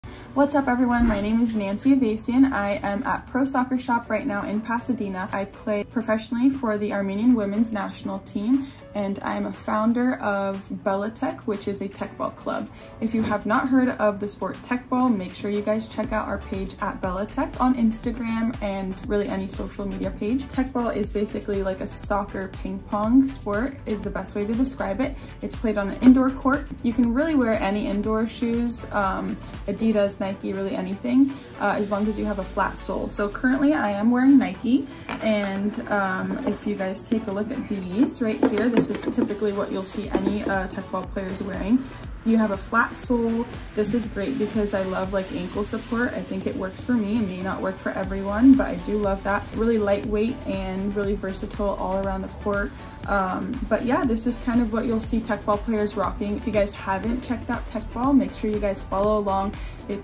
Nancy Avesyan - Nike indoor shoes for Teqball
Nancy Avesyan, from the Armenian National Soccer team talks Teqball and Nike indoor soccer shoes.

The Nike Zoom Superfly 9 academy indoor shoe is great for high-performance indoor soccer and Teqball. With their lightweight design and Nike technology in the heel for responsive cushioning, these shoes are made to give the player the most speed and agility on the field.

The non-marking rubber outsole is made for indoor surfaces and gives great grip for quick cuts and turns during intense Teqball games. The Superfly 9 has a unique look that is great for Teqball players who want to make a statement on the field.

With the Nike Zoom Superfly 9 academy indoor shoe, you can take your indoor soccer and Teqball games to the next level. get yours at prosoccer.com